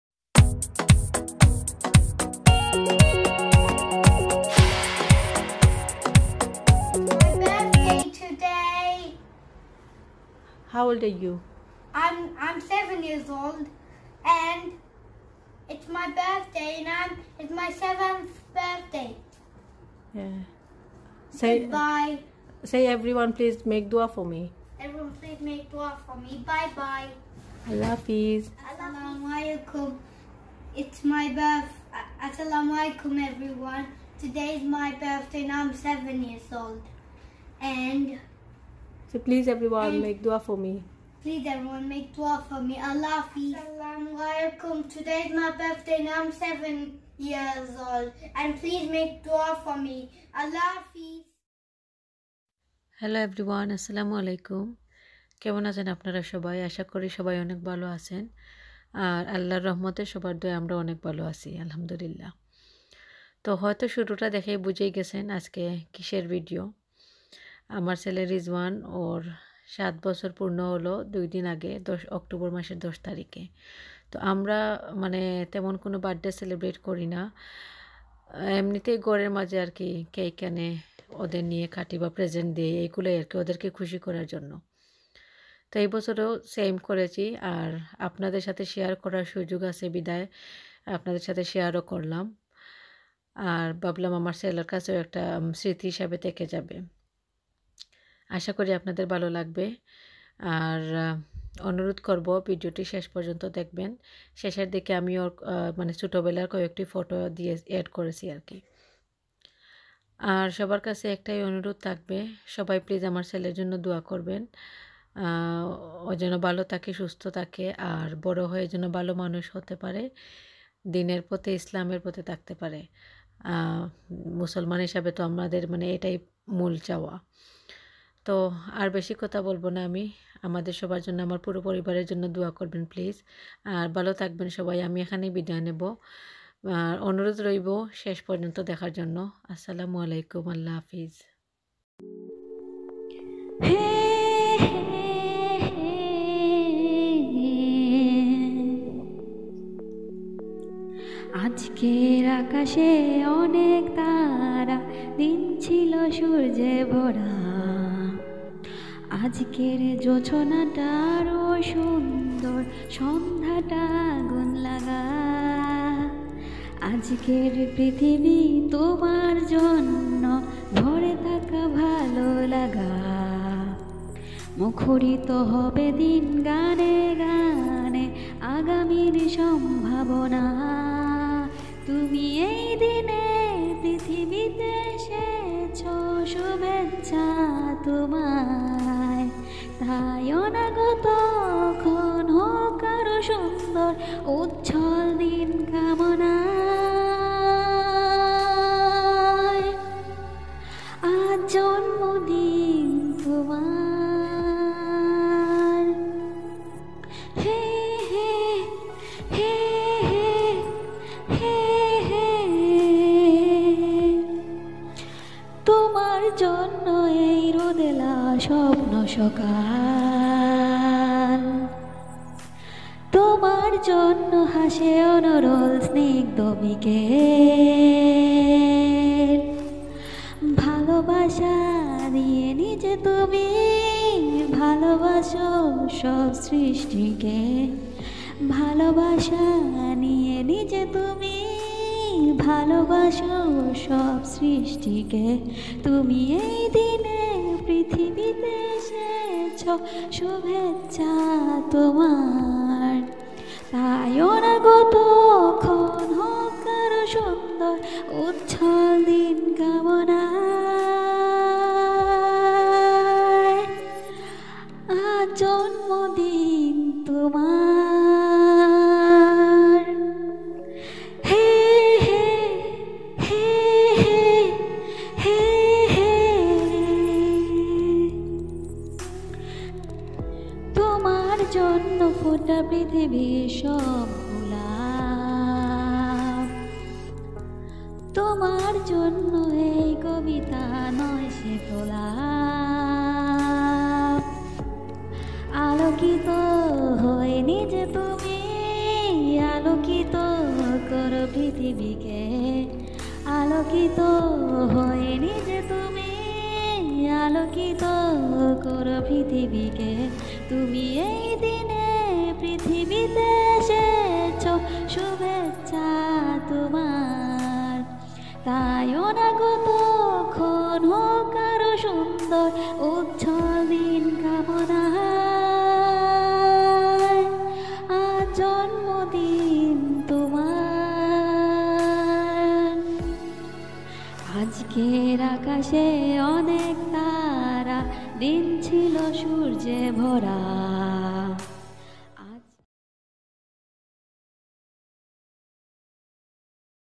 Happy Birthday Rizwan || Birthday vlog || Family Fun
Assalamualikum. Hope you guys fine.Today I upload very special vlog it's my son's 7th birthday.
If you like my video please support me to go ahead. Thank you.